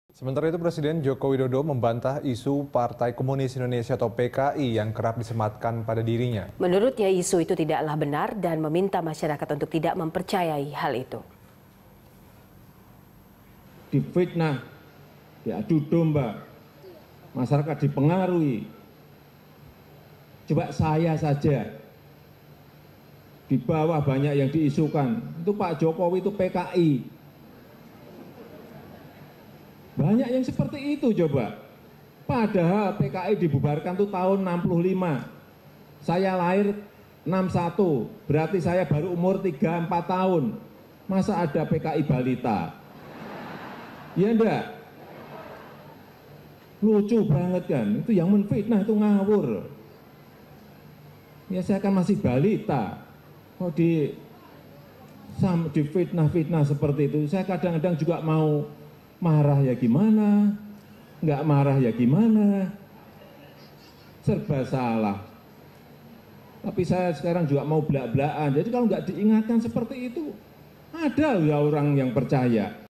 Presiden Jokowi Geram Dirinya Dikaitkan dengan PKI
Presiden Joko Widodo membantah isu Partai Komunis Indonesia (PKI) yang kerap disematkan pada dirinya. Menurutnya isu itu tidaklah benar dan meminta masyarakat untuk tidak mempercayai isu tersebut.

Facebook.com/BeritaSatuTV
Youtube.com/BeritaSatu